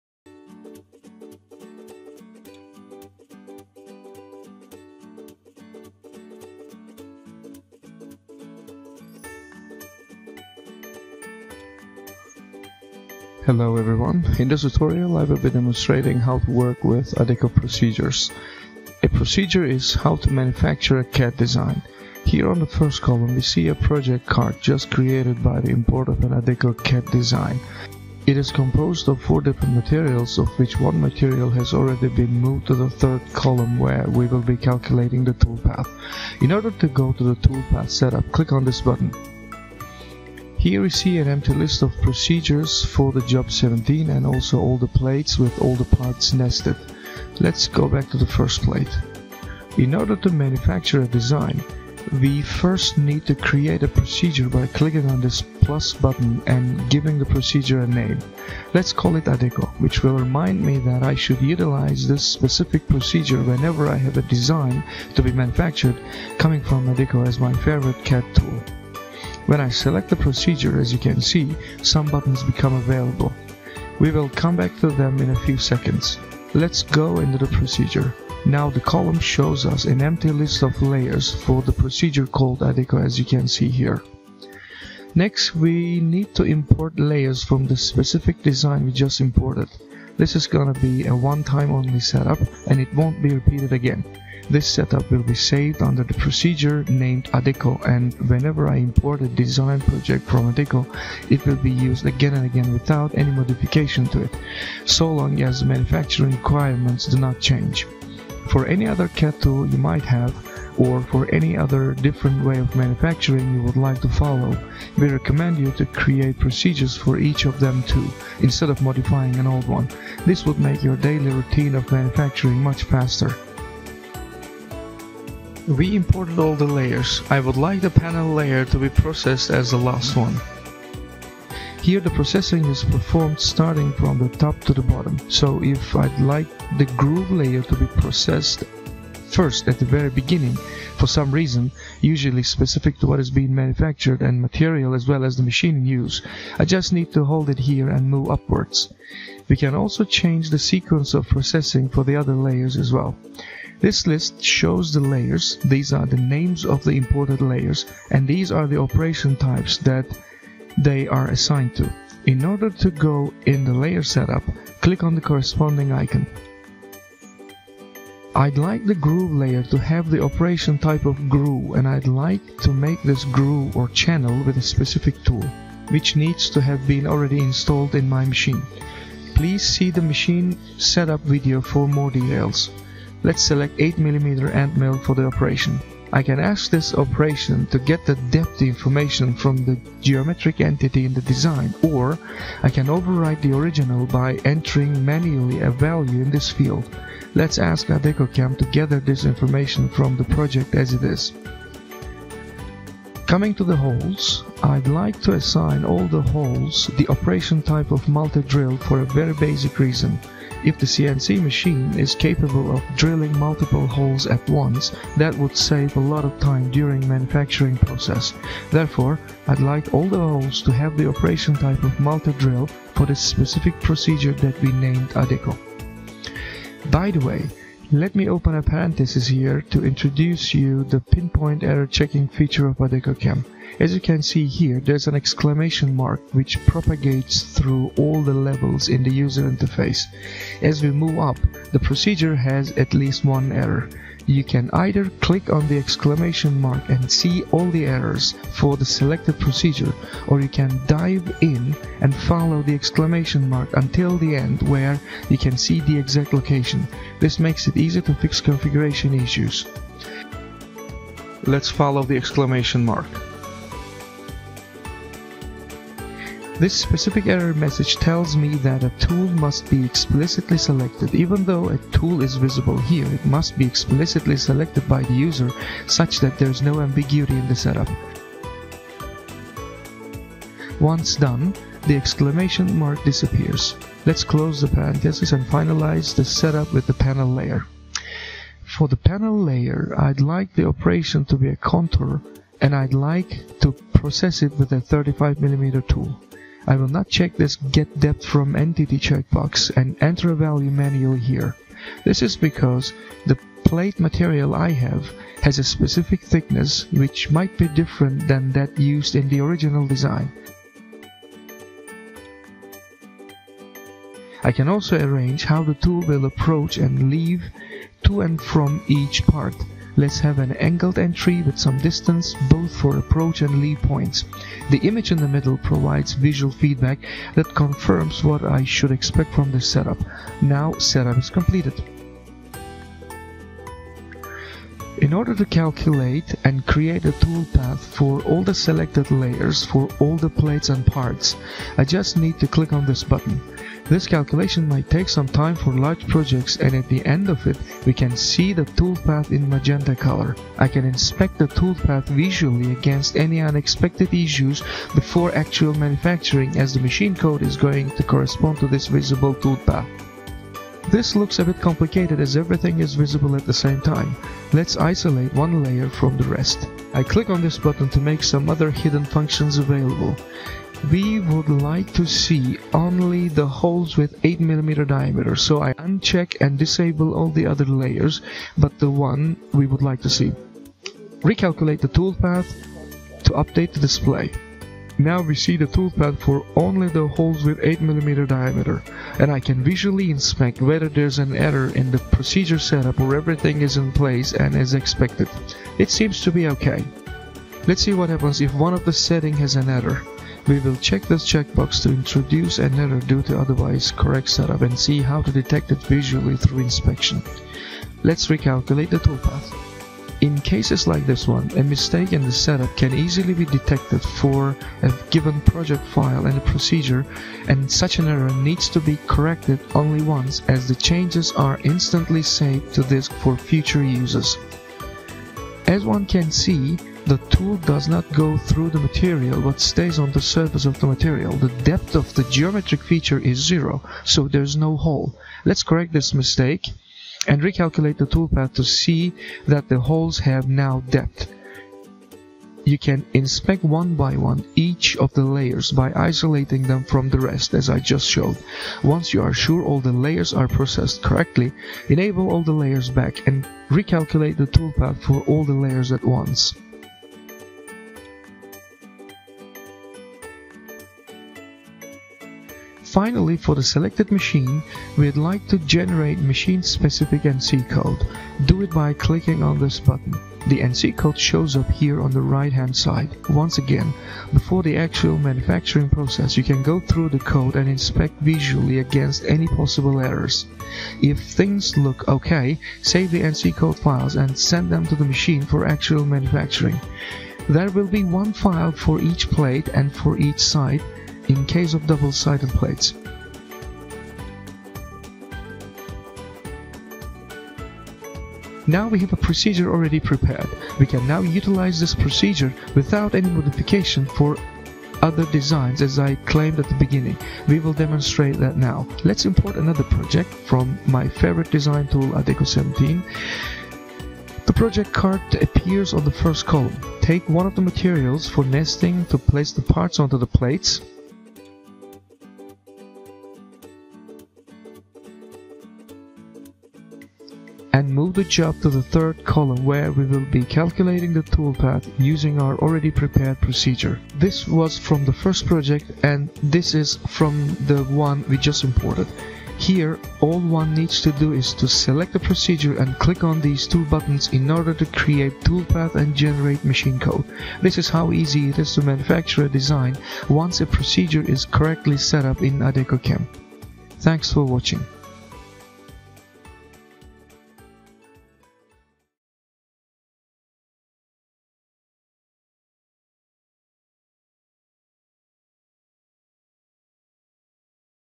adekoCAM - Working with Procedures
- Creating a Procedure
- Importing Layers
- Assigning Operation & Tools
- Pin-point Error Tracking
- Re-using Procedures